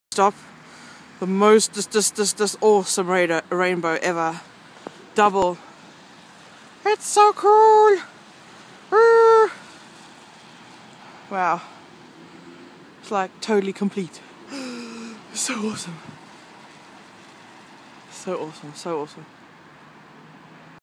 Awesome double rainbow is AWESOME!
Driving over the North Circular last night and saw the most awesome double rainbow!!! God is awesome!